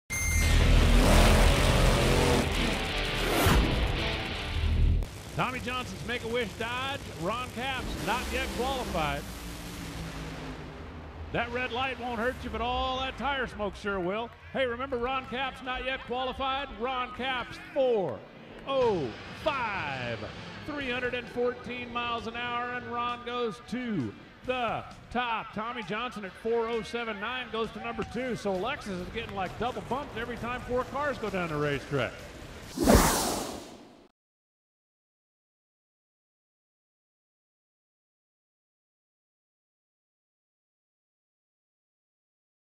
Ron Capps goes to the top Friday at the 4Wide
Napa Auto Parts Funny Car driver Ron Capps goes to the top after the second qualifying session on Friday at the NHRA Four Wide Nationals at zMAX Dragway in Charlotte.